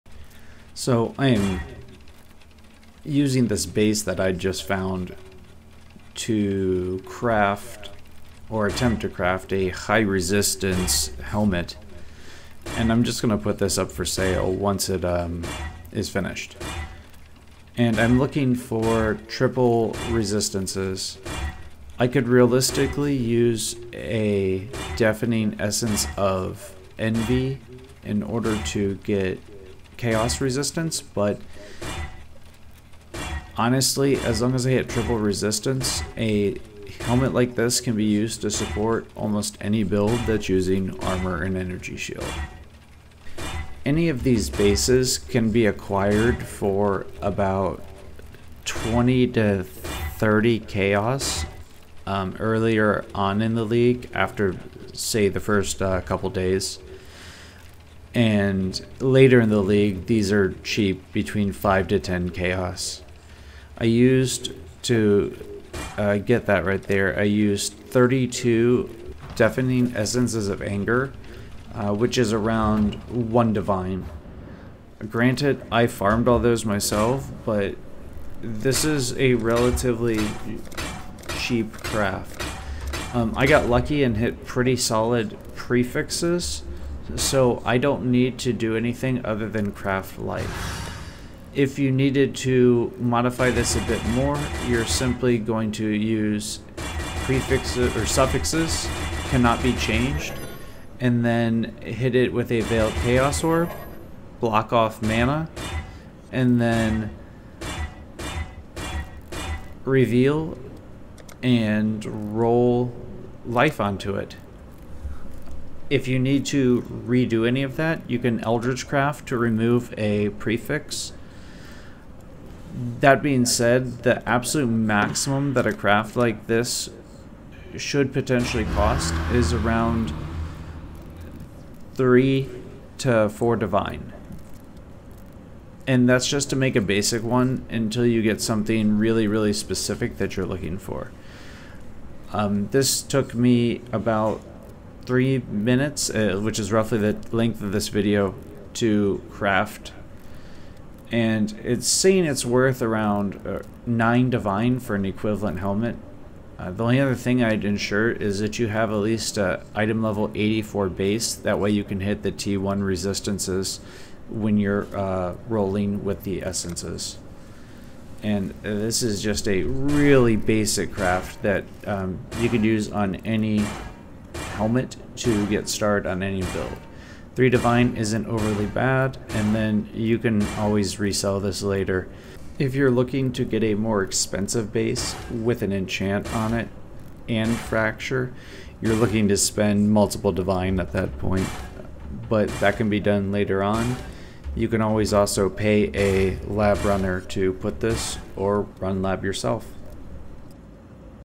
Cheap Generic Helmet Craft to Support Most Builds
Brief crafting of a helm that can be used to support almost any build:

Base: ilvl 84 Helmet with t1 fractured resistance (5-30 chaos)

Crafting Process
Essence spam cost: 1-2 Divine (RNG dependent)
Using a resistance essence looking for a 3rd resistance
Suffixes cannot be changed: 2 Divine
Veiled chaos Orb
Craft: Mana to block (This is optional mana + life regeneration isn't bad)
Unveil: Select best mod
Craft Life
Fill empty prefix with a slam.
(You can redo the prefixes later if you want something better using eldritch currency.)
If you are after an enchanted version, this will cost more. It is normally between 1-20 divine for a fractured helmet with a lab enchant. This depends on how popular the build is.